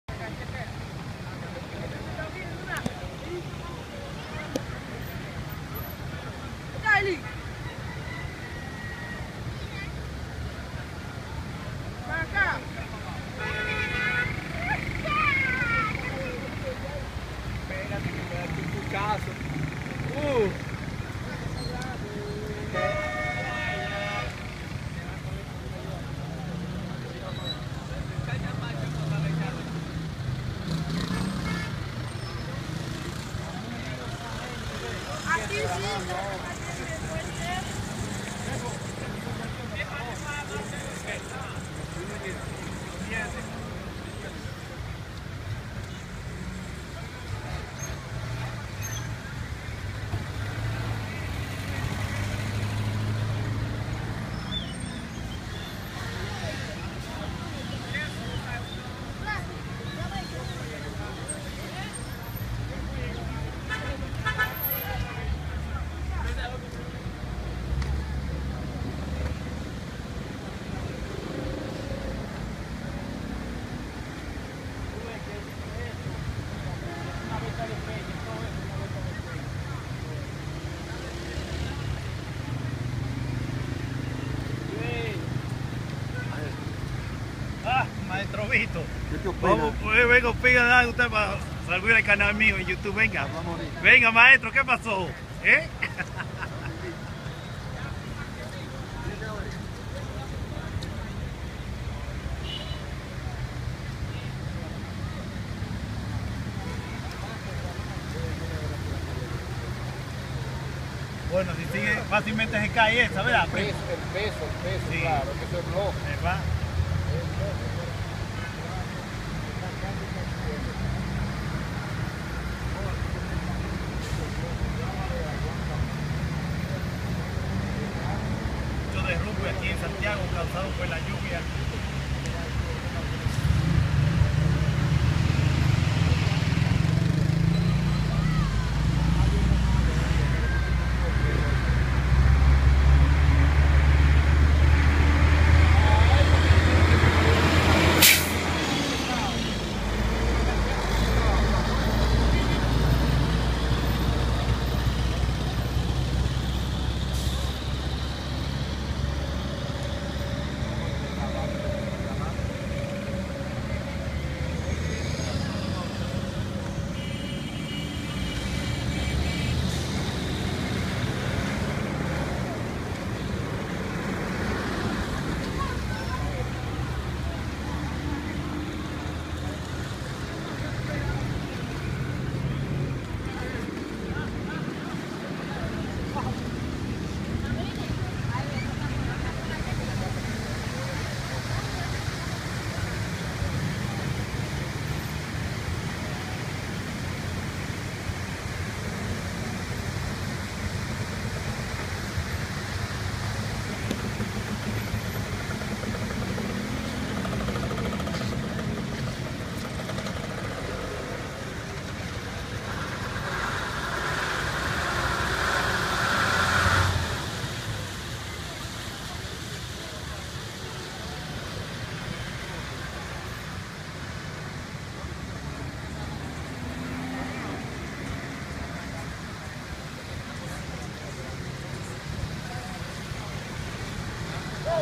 Sigue lo derrumbe de casa en Santiago por causa de la lluvia en República Dominicana
Inundaciones en Santiago República Dominicana